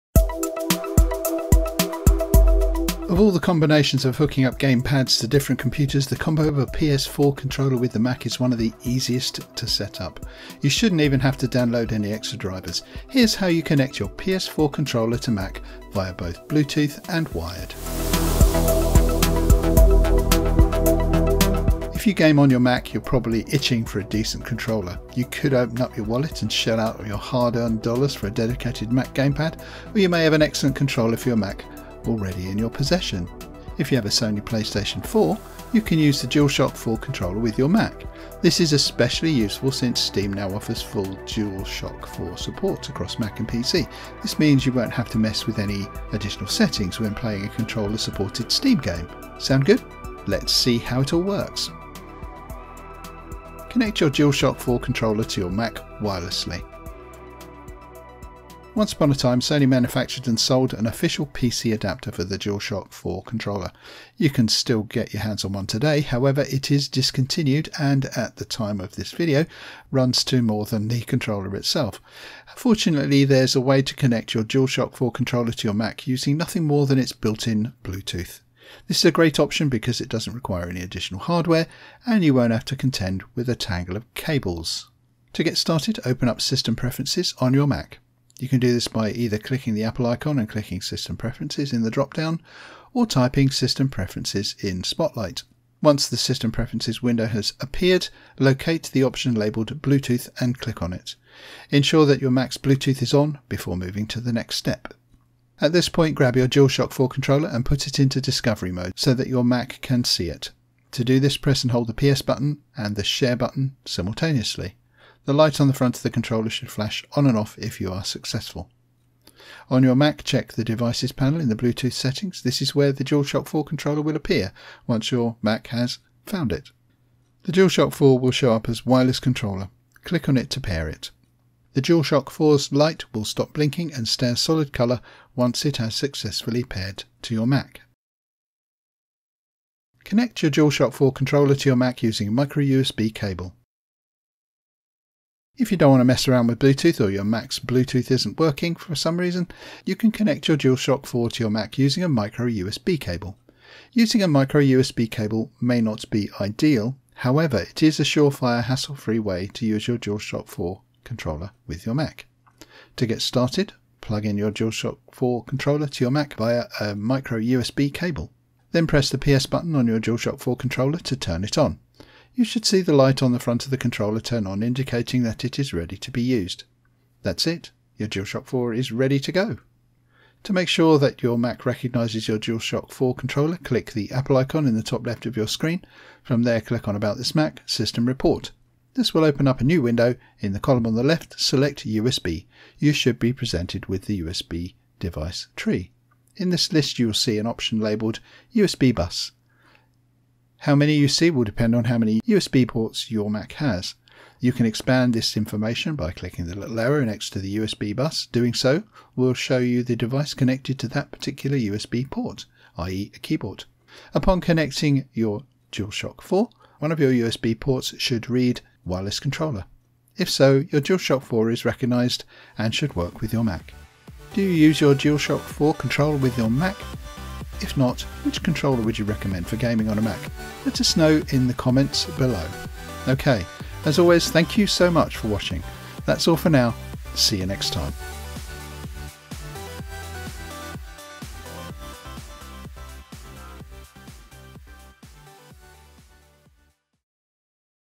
Connect your PS4 Controller to Mac (Bluetooth/USB)
Of all the combinations of hooking up gamepads to different computers, the combo of a PS4 controller with a Mac is one of the easiest to set up. You shouldn't even have to download any extra drivers! Here's how to connect your PS4 controller to Mac, both via Bluetooth and wired.

Script by Ryan Lynch